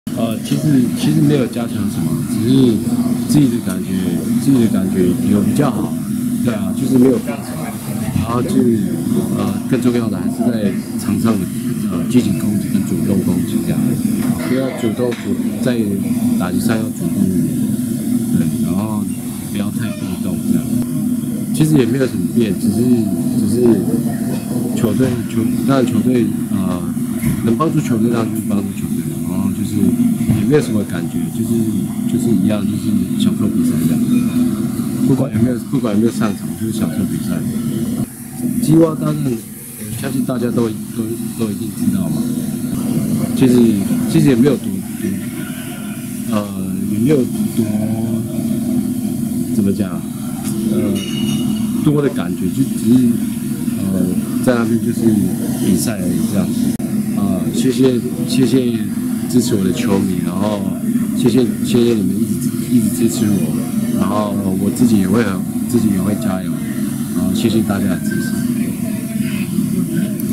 林子偉享受比賽 「打擊要主動一點」
對於球迷的支持～子偉表示非常感謝！

【未經同意請勿使用、翻拍、下載或嵌入】